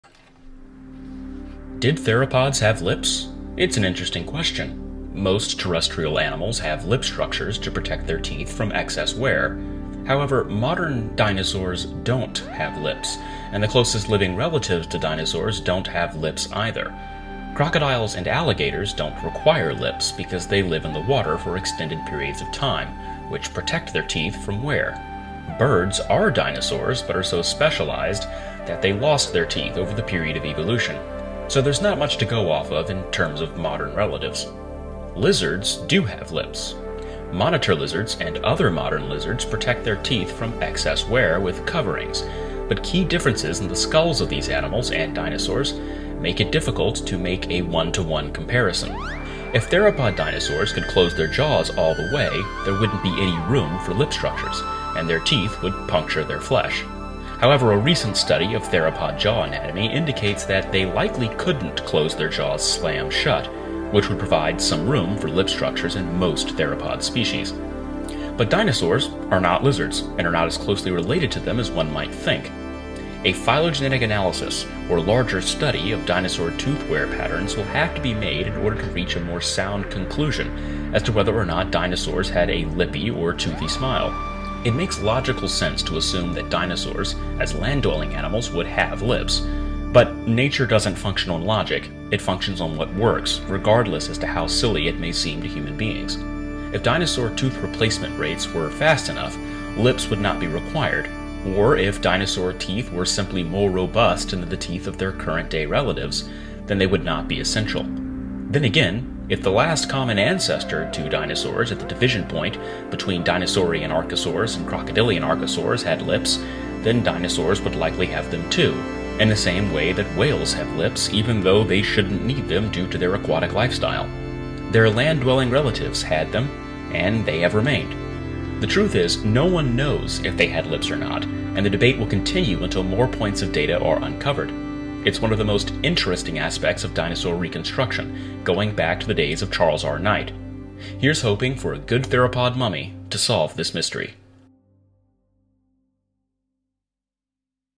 Lips on Theropods? A Toothy Debate
It's probable that dinosaurs didn't have the infamous toothy grins we've seen in Hollywood movies. However, despite many recent studies there is still some debate as to whether or not these great animals had lips to cover their teeth!